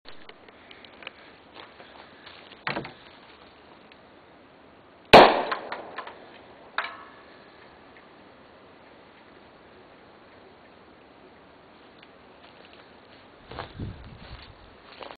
Parach_eject_Solaris_H_1SM_short.MPG
Parachute ejection ground test for the Solaris_H_1SM HRM experimental rocket.